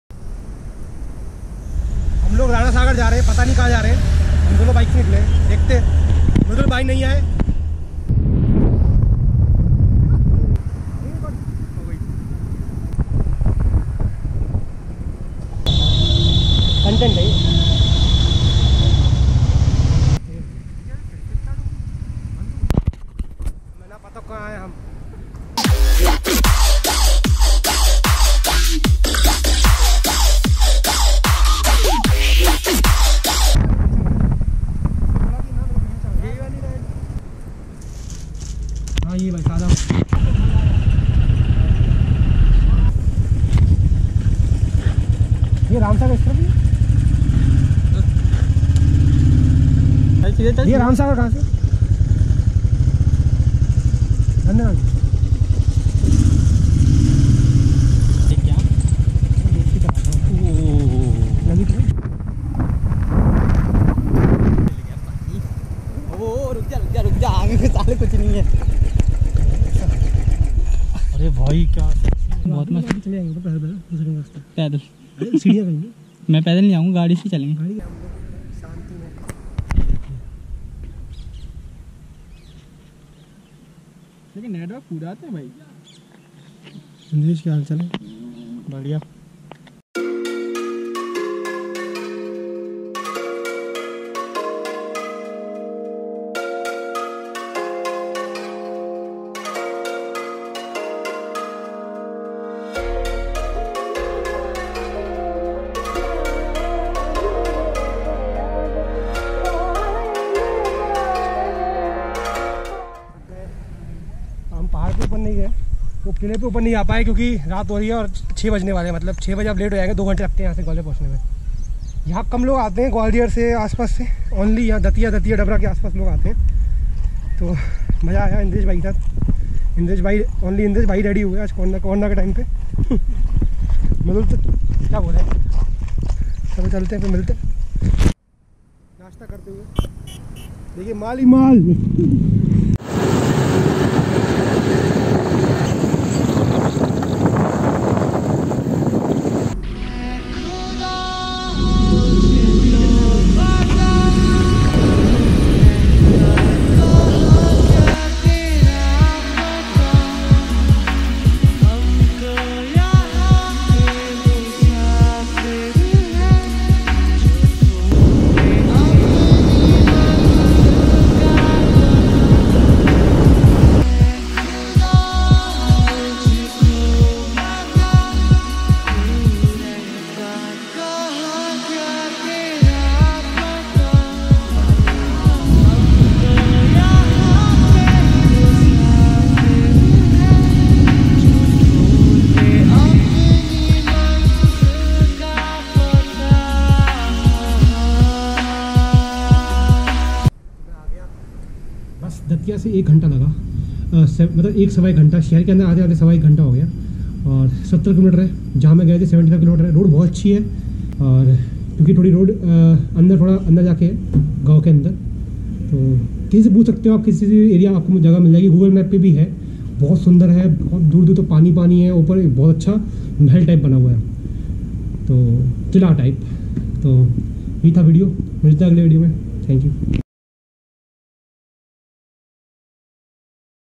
Ramsagar Dam || Badoni Fort , Datia
Ram Sagar, Datia

a great place to escape into nature

there was a big " Taal " here.. the main beauty was the Fort on the top of a hill, from the top you can see the beautiful blue water, and landscape of Rocky hills...